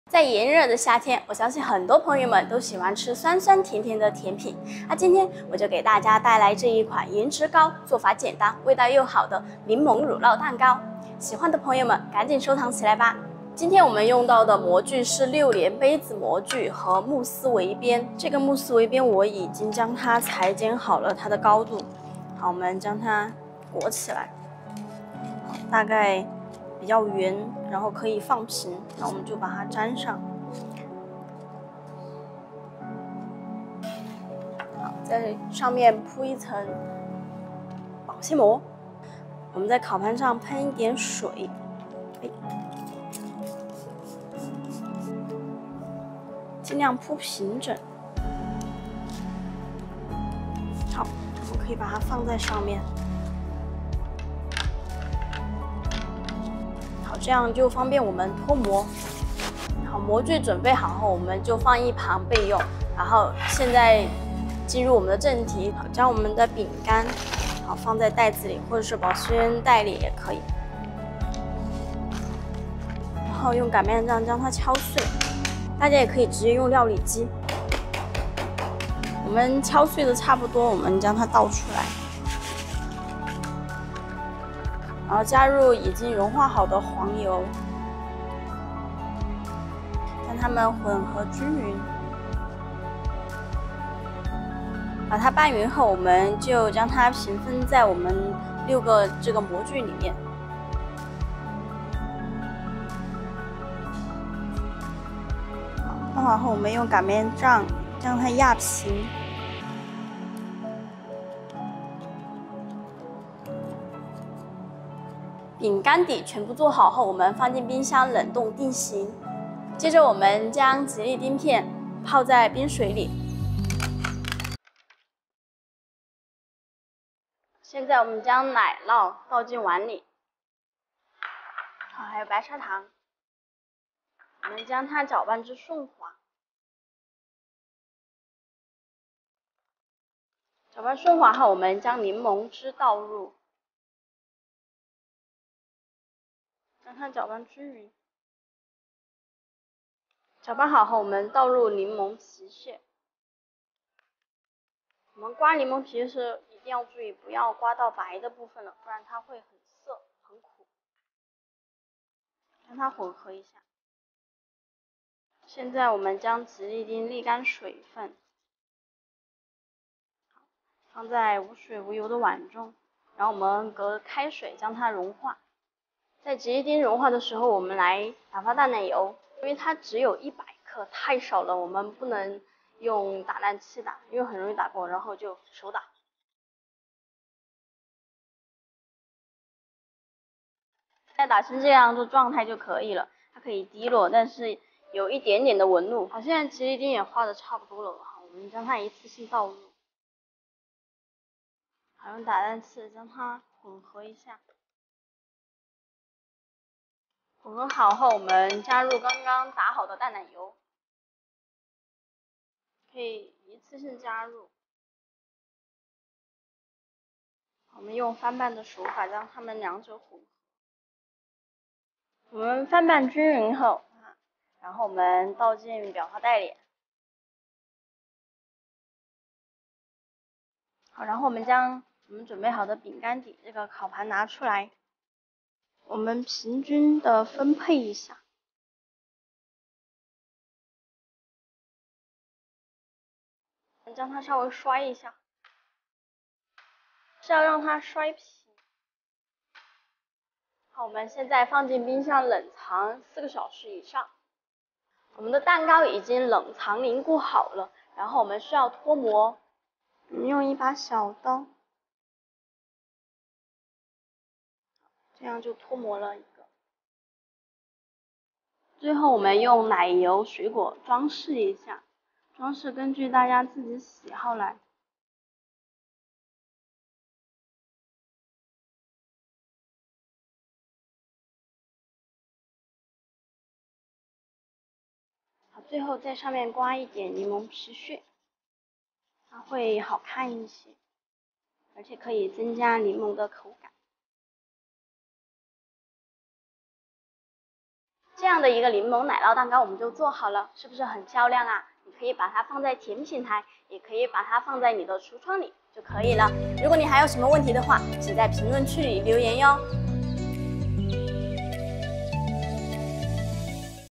最适合夏天吃的柠檬乳酪蛋糕！无需烤箱，口感清爽，建议收藏
"视频制作过程中，我们特别强调了对动物参与者的保护和福祉，绝无涉及虐待行为。"